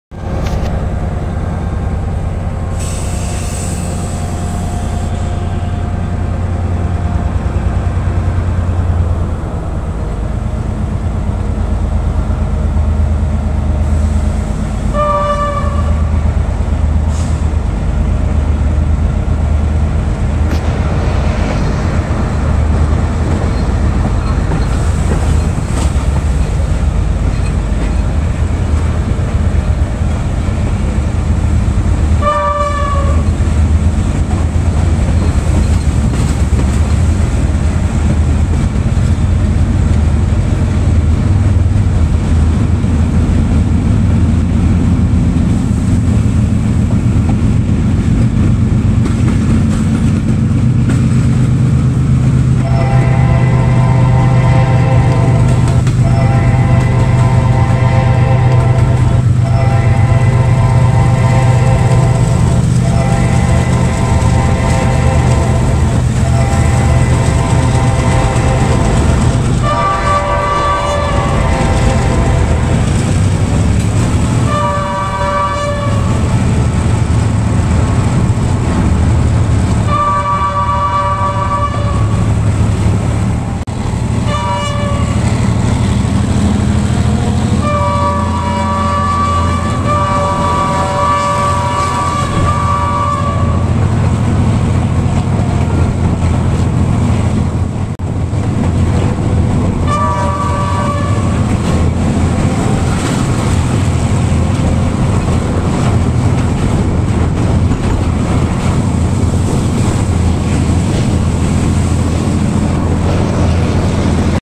السباق الذي يبحث عنو الملايين🤔
حصريا اول مره سابق بين اقدم قطارفي مصر كندي واحدث قطار جنرال 🔥🚀وحلي تحيا بين السواقين